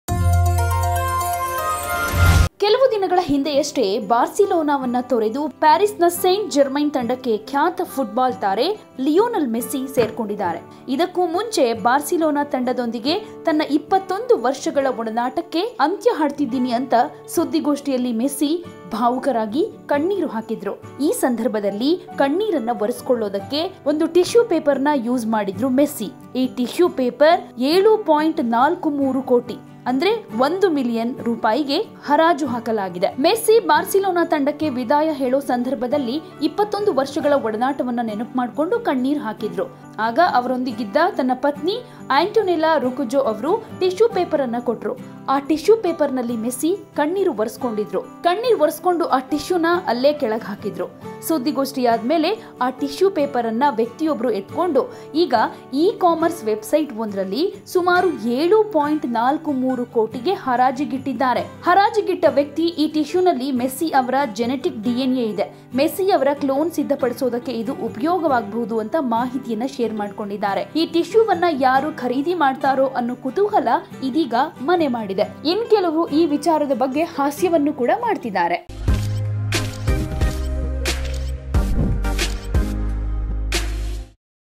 ಅಬ್ಬಾ!!ಮೆಸ್ಸಿ ಕಣ್ಣೀರು ಒರೆಸಿ ಬಿಸಾಡಿದ ಟಿಶ್ಯೂ ಬೆಲೆ ಕೋಟಿ ಕೋಟಿ ರೂಪಾಯಿ | Oneindia Kannada
ಬಾರ್ಸಿಲೋನಾ ತಂಡದೊಂದಿಗಿನ ತನ್ನ 21 ವರ್ಷಗಳ ಒಡನಾಟಕ್ಕೆ ಅಂತ್ಯ ಆಡಿದ ಸುದ್ದಿಗೋಷ್ಠಿಯಲ್ಲಿ ಮೆಸ್ಸಿ ಕಣ್ಣೀರು ಹಾಕಿದ್ದರು. ಈ ವೇಳೆ ಕಣ್ಣೀರು ಒರೆಸಿಕೊಳ್ಳಲು ಬಳಸಿದ ಟಿಶ್ಯೂ ಪೇಪರ್ 7.43 ಕೋಟಿ(1 ಮಿಲಿಯನ್)ಕೋಟಿ ರೂಪಾಯಿಗೆ ಹರಾಜು ಹಾಕಲಾಗಿದೆ.

Tear-soaked tissue used by Lionel Messi during Barcelona farewell up for sale at $1 million
Oneindia kannada